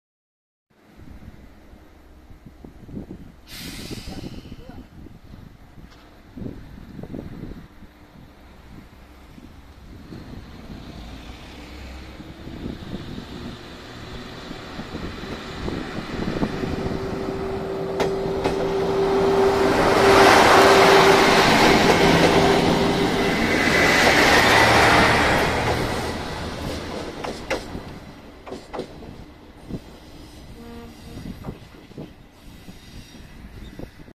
阿南駅をゆっくりと発車する、1500型気動車。
こちらは、去年に撮影した、1500型気動車の発車動画です。

こちらは、阿南駅の外にある、駐輪場付近で撮影しました。

2両編成の1500型気動車が、ゆっくりと阿南駅を発車して行きます。

少しドアップで撮影したので、迫力はあるかと思います。

先頭には、あの2編成しか存在しない、1500型のイメージチェンジ車両、1500型7次車も一緒に運用に入っています。

阿南駅を発車する、1500型気動車の動画を、どうぞご覧下さい。